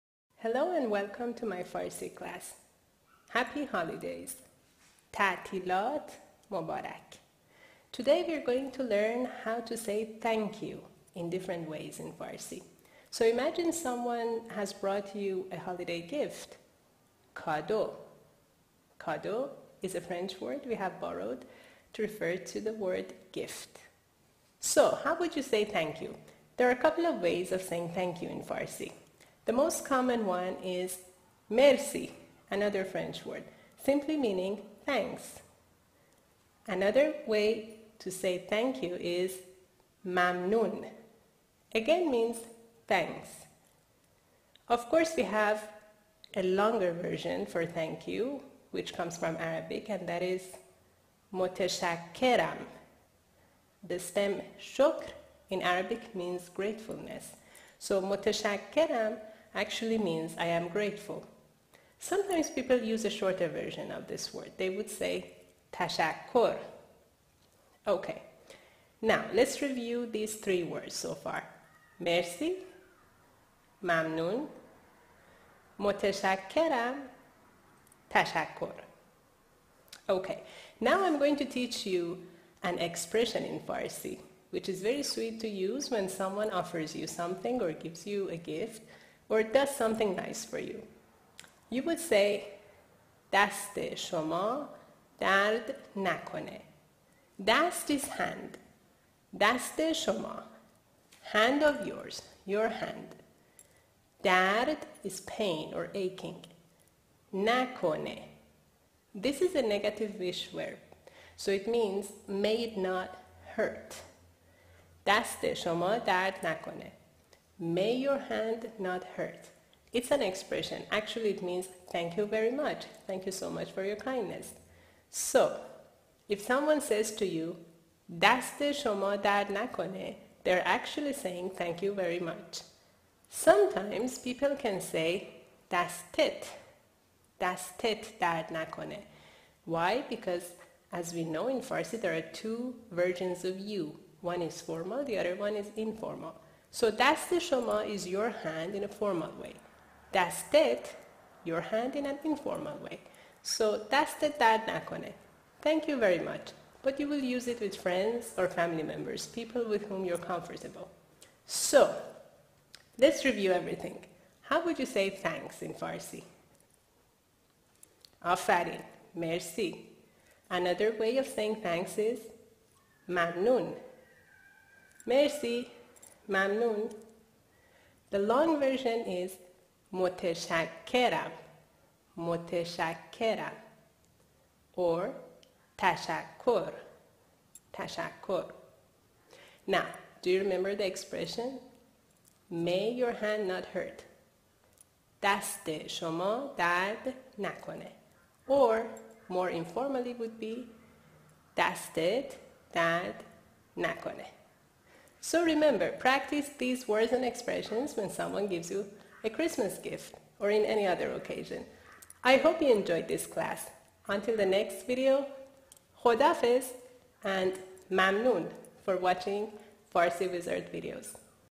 Farsi / Persian Lesson: "Thanks!" (57)
Learn how to say "Thanks" or "Thank you" in different ways in Farsi / Persian :-)